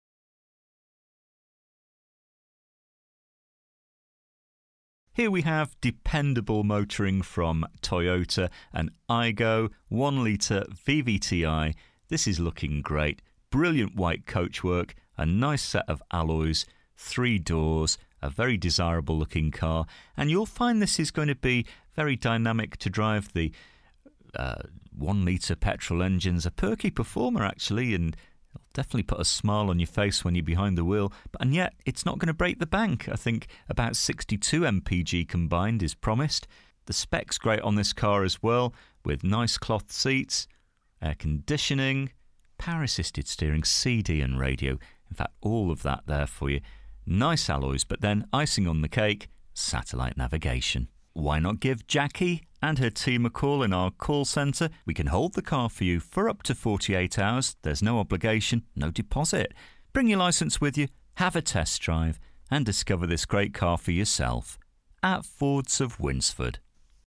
TOYOTA AYGO HATCHBACK (2011) 1.0 VVT-I GO 3DR - FE11XTJ
DETAILS:
First registered: 17/03/2011
Current mileage: 46620
Registration: FE11XTJ
Colour: WHITE

FEATURES:
front electric windows, remote central locking, cloth interior, all round airbags, air conditioning, pas, abs, alloy wheels, solid paint, cd, satellite navigation, parcel shelf present, hand book presen

This used car is for sale at Fords of Winsford Car Supermarket in Cheshire, UK